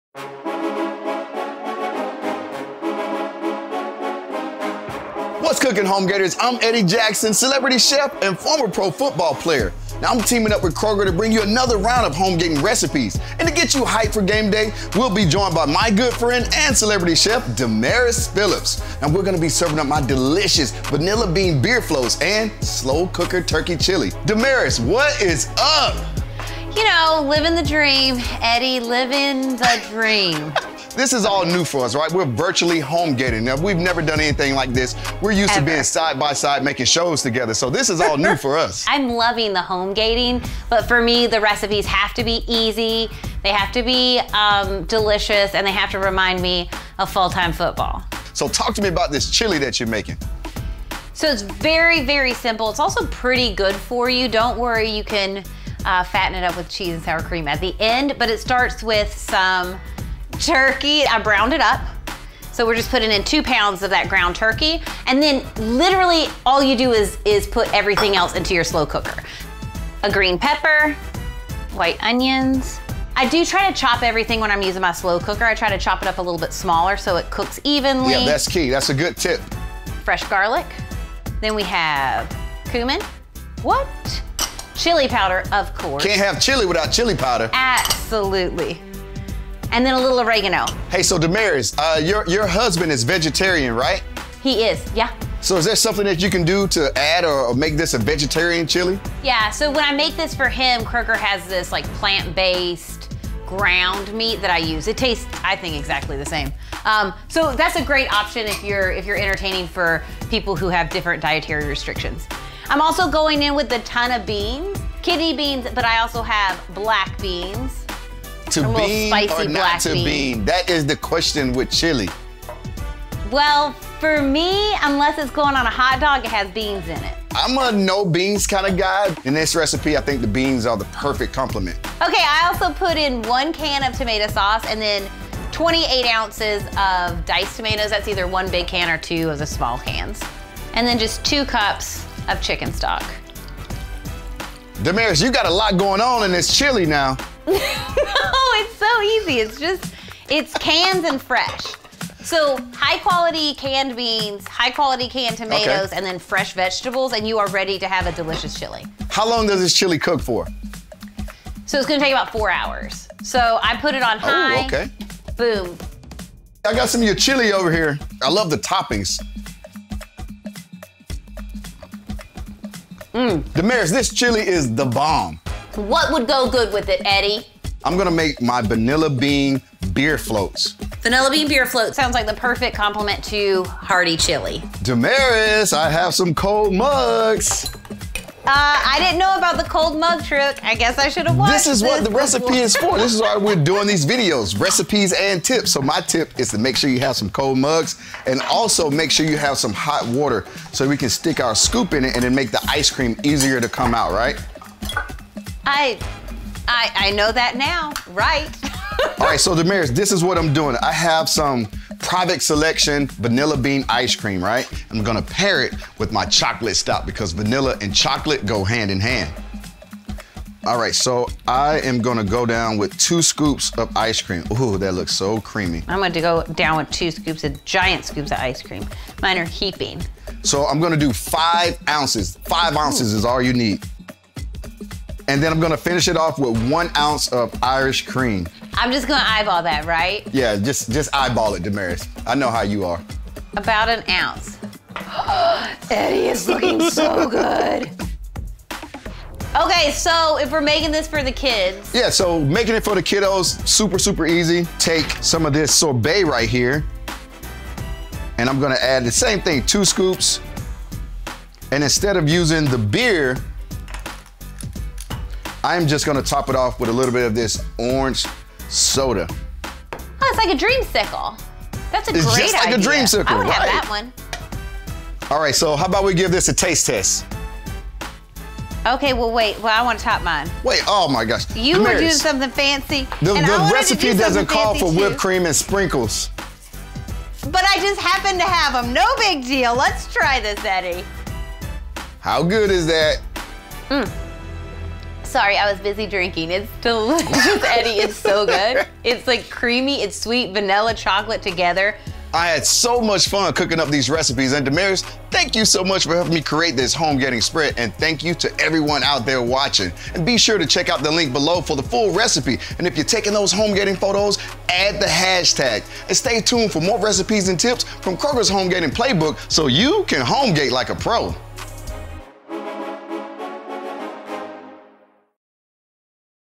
How to Make Vanilla Bean Beer Floats | Game Day Recipes | Fry's
We are back with former pro football player and celebrity chef Eddie Jackson! Watch as he breaks down his Vanilla Bean Beer Float recipe on gameday with an assist from Damaris Phillips.